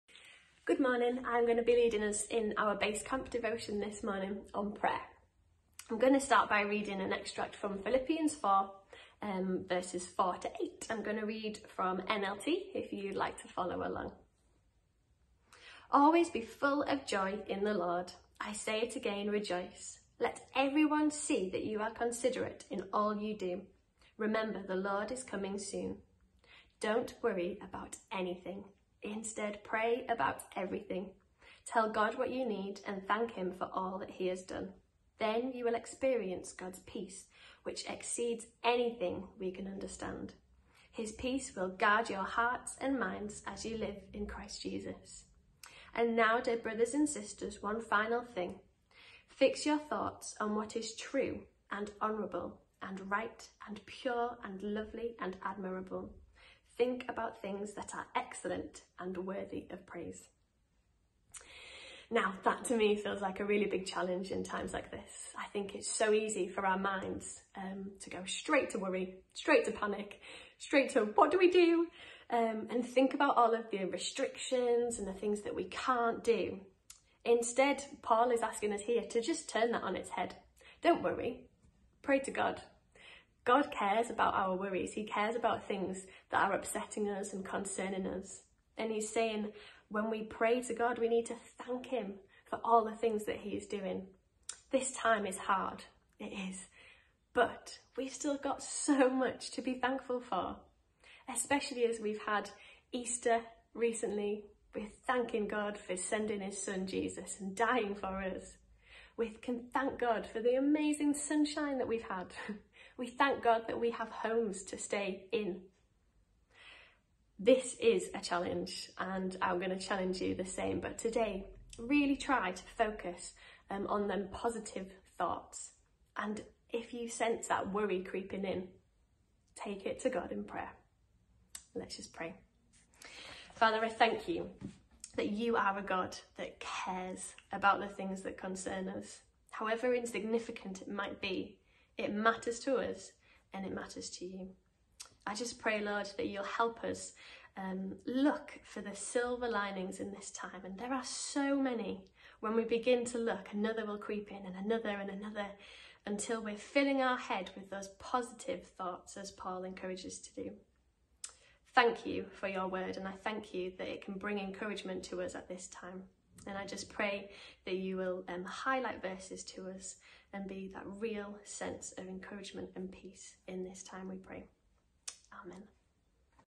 Base Camp Devotion: pray. 22 April 2020
Here's Amy Spence with this morning's Base Camp Devotion on 'pray'.